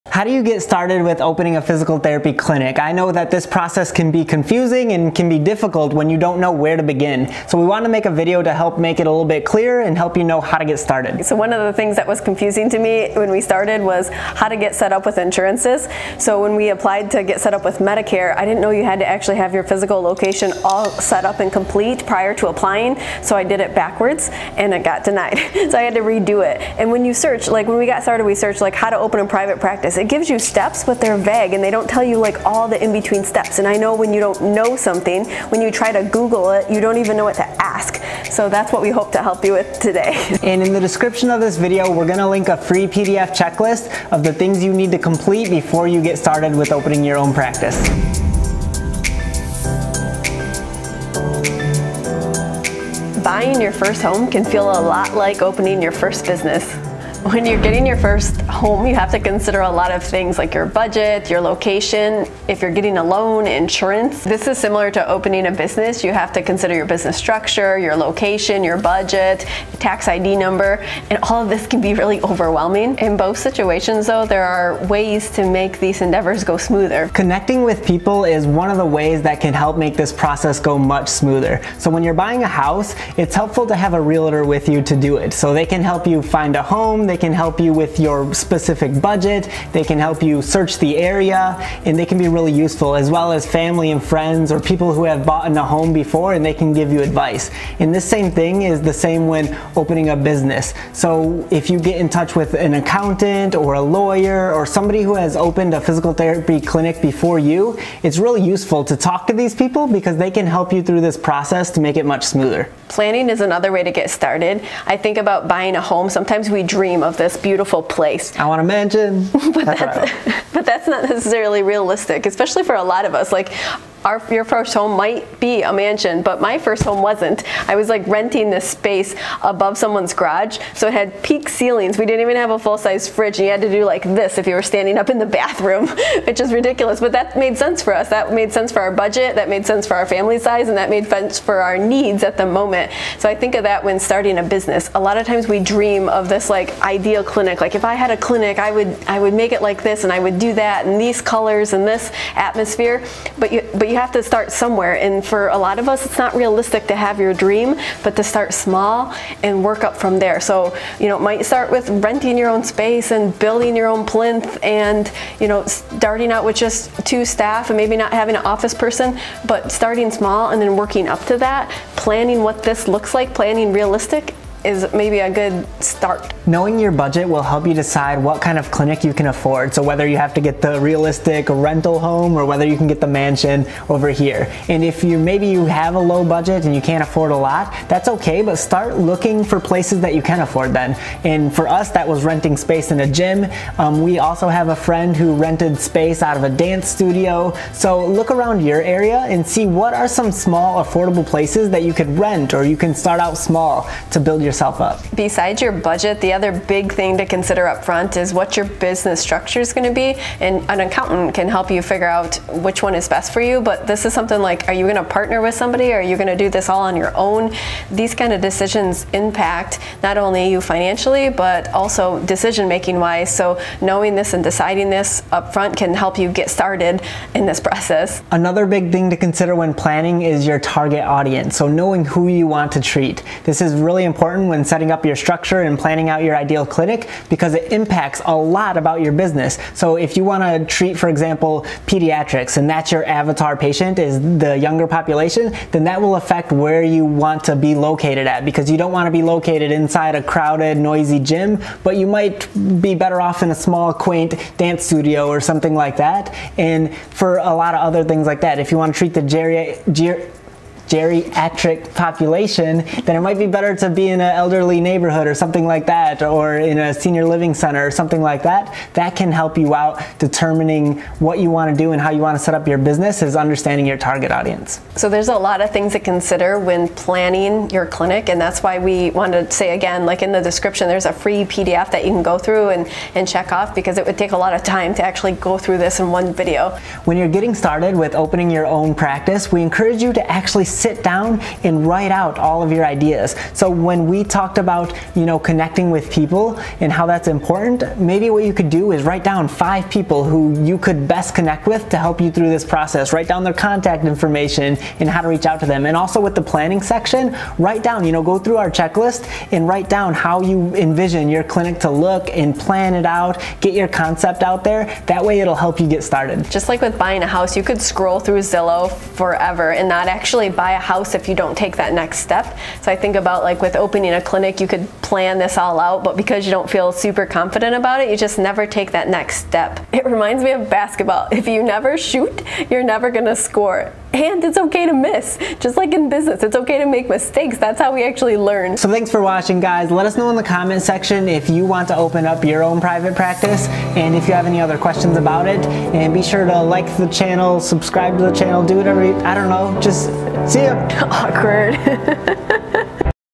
How to Get Started With Opening a Physical Therapy Clinic?
How to get started with opening a physical therapy clinic? This video is for people trying to start a private practice. It goes through how to get started and gives a plan of where to begin.